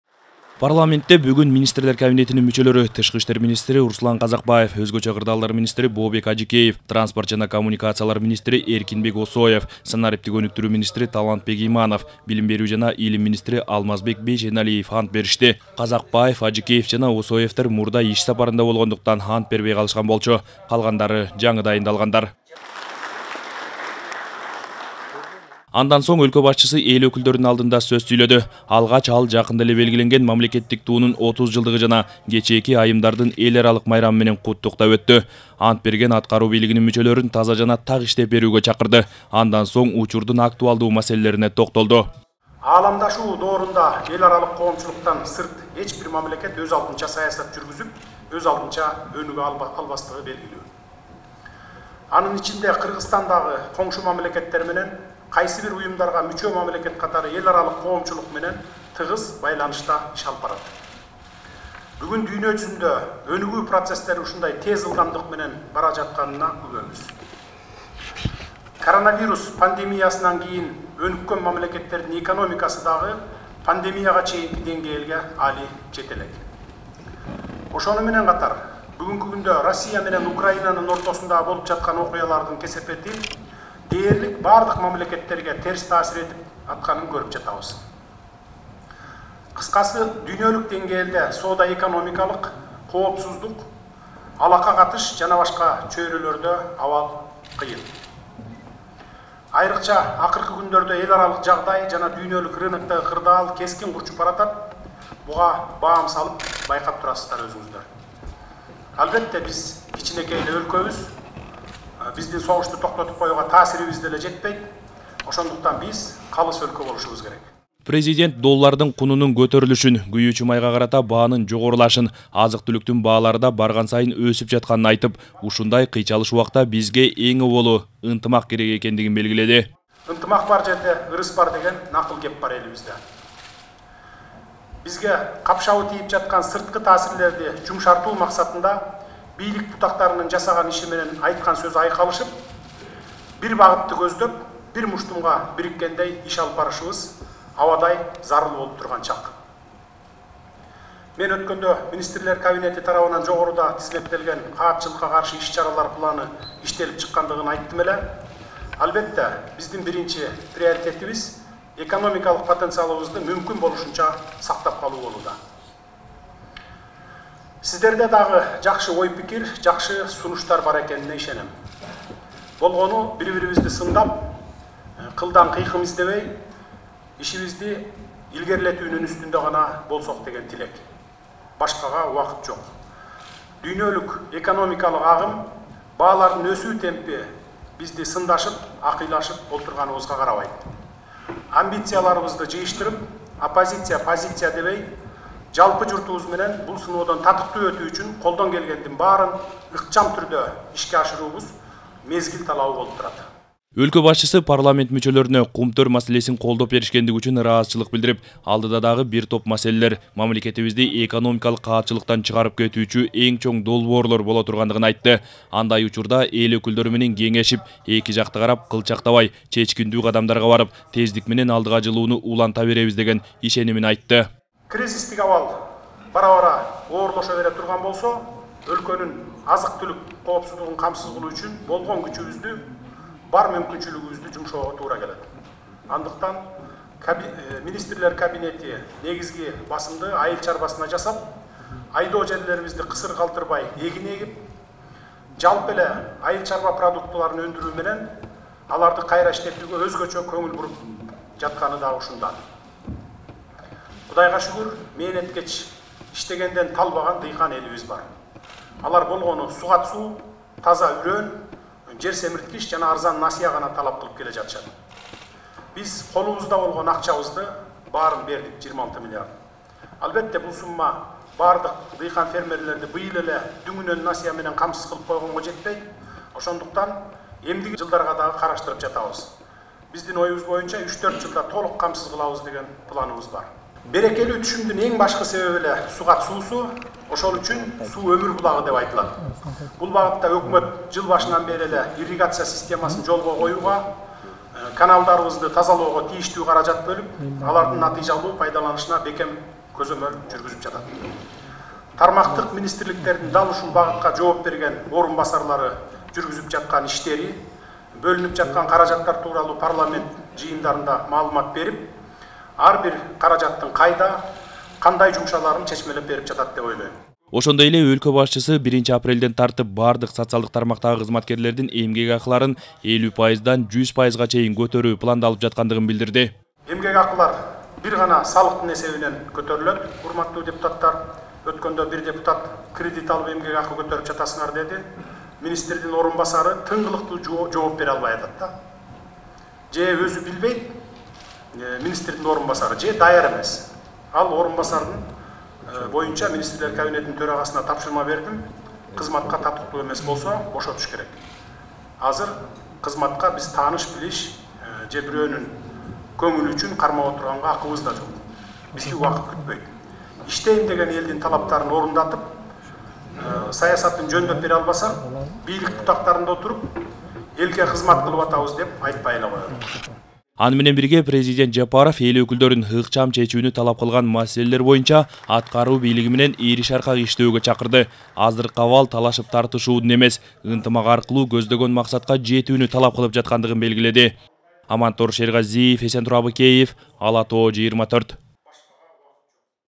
Садыр Жапаров Министрлер Кабинетинин жаңы мүчөлөрүнүн ант берүүсүнө катышты
Бизге капшабы тийип жаткан сырткы таасирлерди жумшартуу максатында бийлик бутактарынын жасаган иши менен айткан сөзү айкалышып, бир багытты көздөп, бир муштумга бириккендей иш алпарышыбыз абадай зарыл болуп турган чак. Мындай пикирин президент Садыр Жапаров бүгүн Жогорку Кеңеште сүйлөгөн сөзүндө билдирди. Мамлекет башчысы Министрлер Кабинетинин жаңы мүчөлөрүнүн ант берүүсүнө катышып, парламентте кайрылуу кылды. Анда ал бири-бирибизди сындап, “кылдан кыйкым издебей”, ишти илгерилетүүнүн үстүндө аракет кылуу учур талабы экендигин белгиледи.